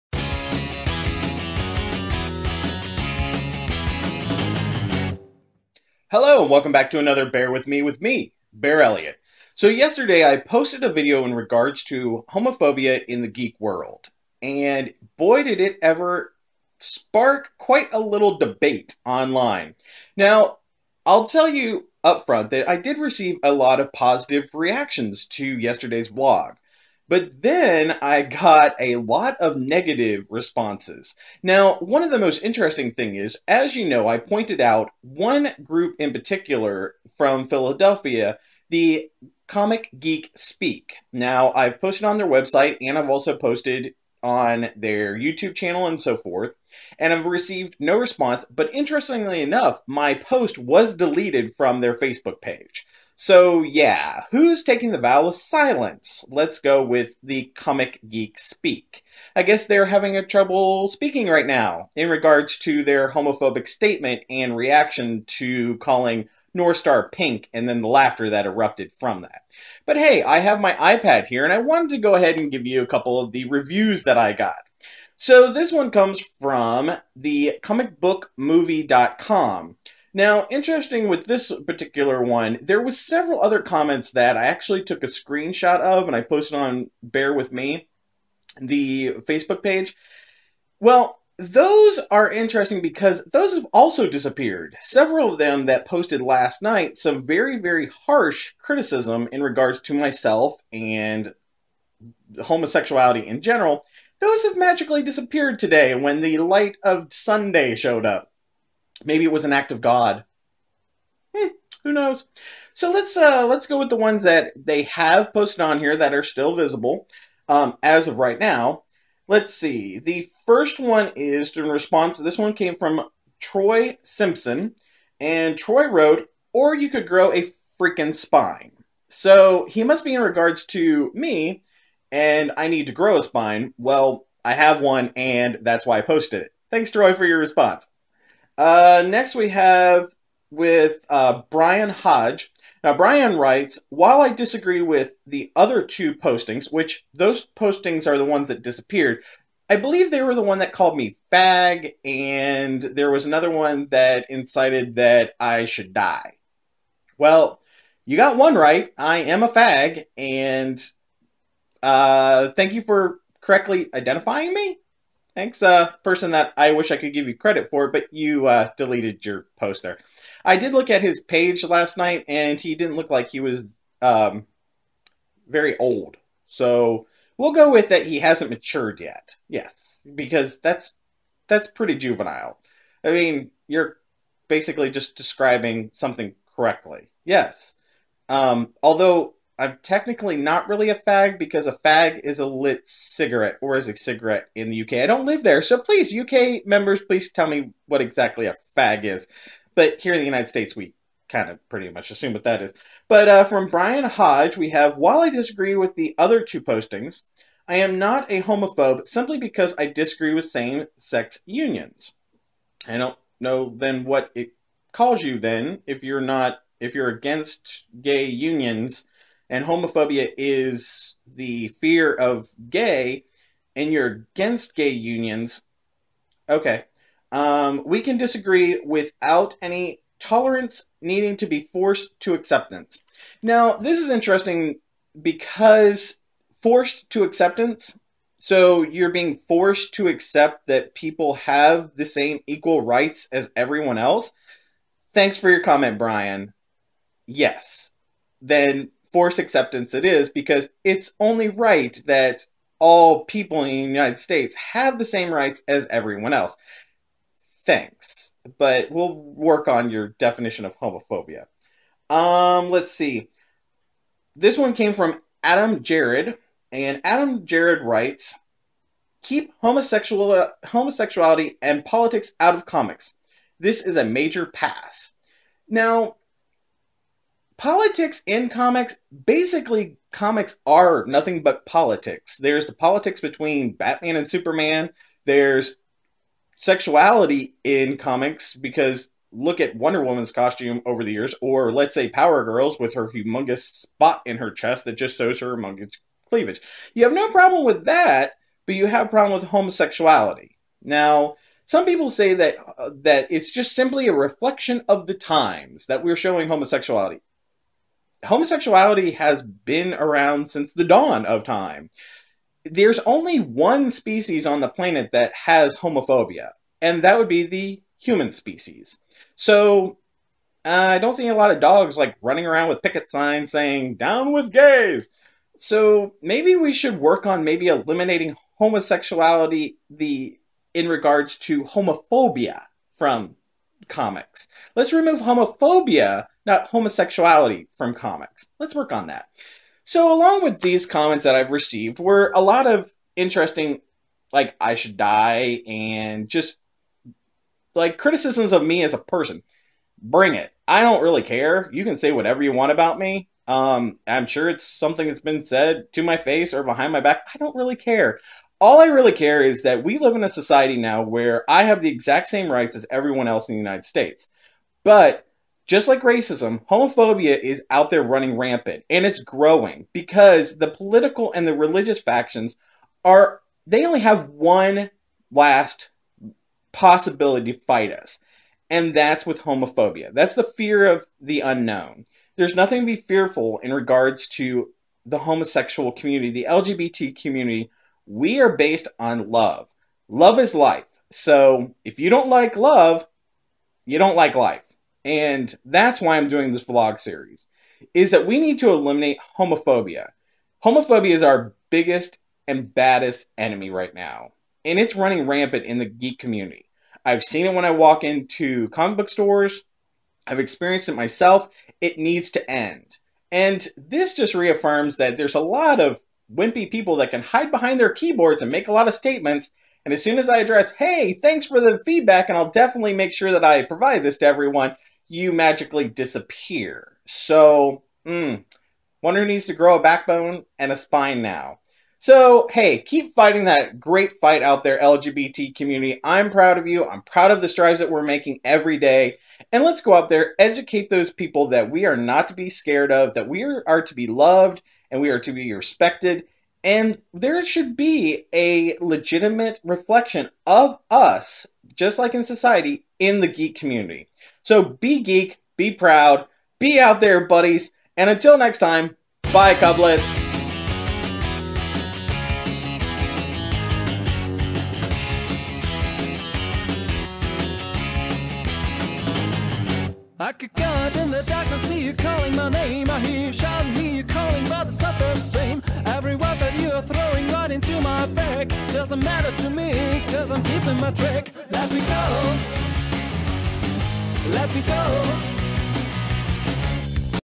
Reactions To Homophobia Vlog
Another day, another string of anti-Gay and Homophobic comments. It was a very interesting the range of responses to my Vlog yesterday I received from the Facebook pages and Sites that I posted on.  There were some wonderful feedback and positive messages of encouragement and I thank them all for that. Then there were the ones that were hate filled and nasty. Here are just a few of those winners I got. Stop Homophobia and remember Love is Life!

BearElliott

BearElliott